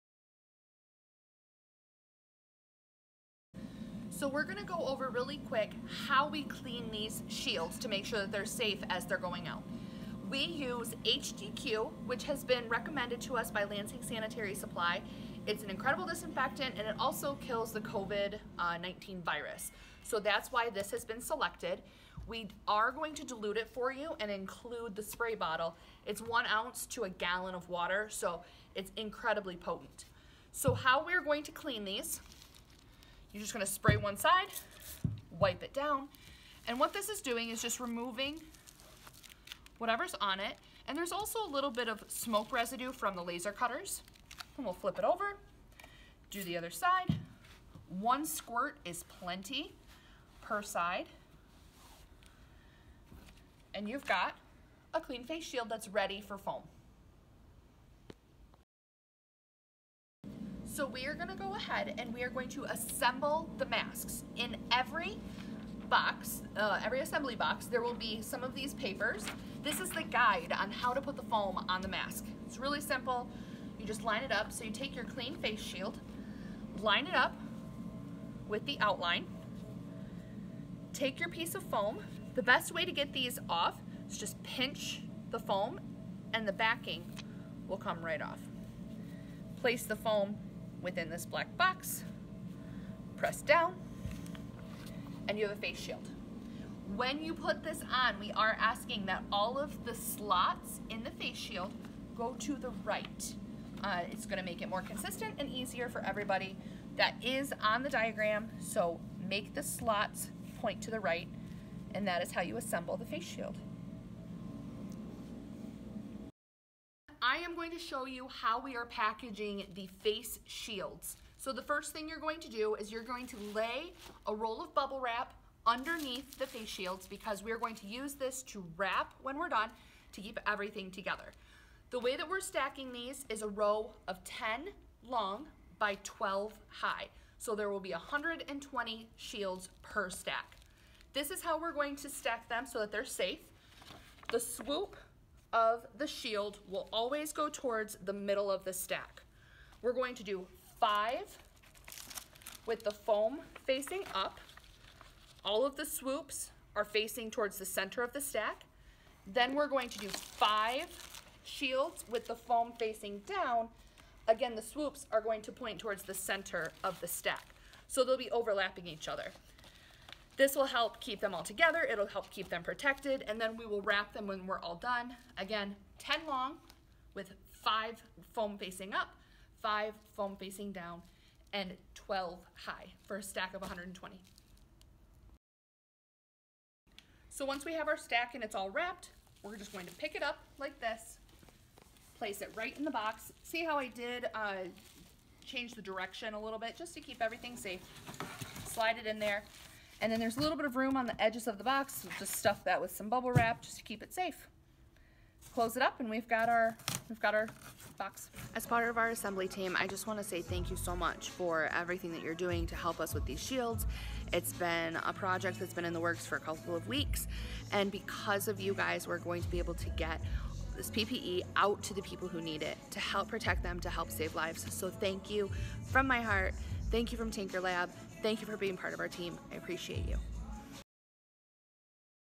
Face Shield Assembly
Tutorial on how to assemble and clean face shields for COVID donations.